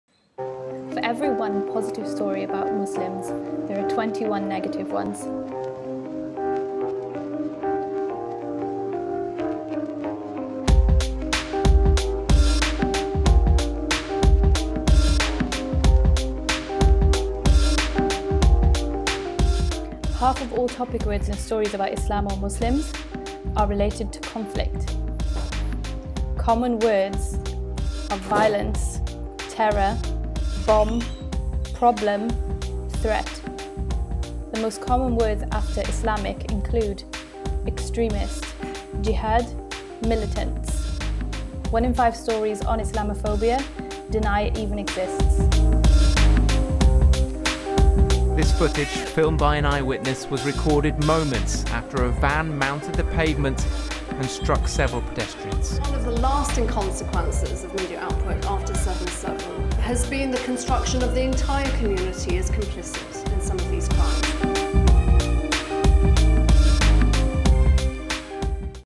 Islamophobia and the Media
Does the media negatively influence the way we see Muslims and Islam?